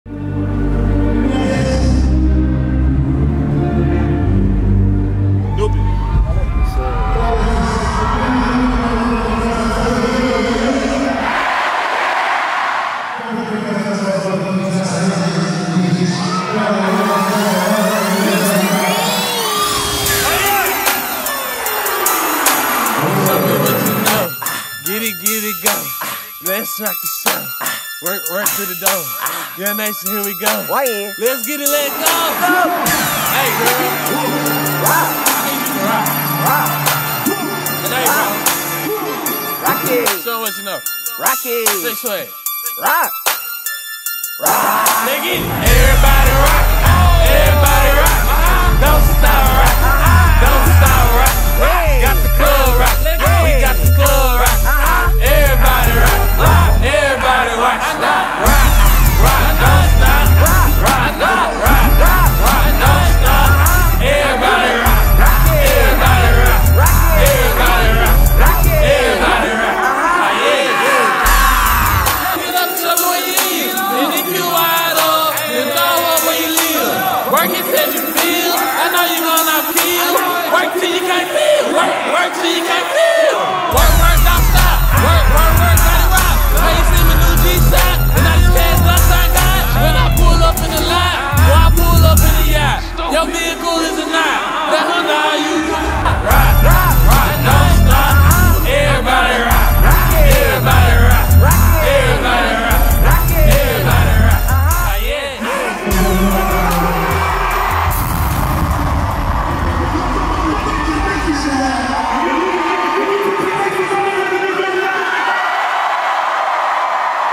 YUNG NATION Live at Don Toliver Love Sick Tour with Tisa Korean
YUNG NATION Live at Don Toliver Love Sick Tour with Tisa Korean (Rare Footage)

Visuals by Hit Stick Kb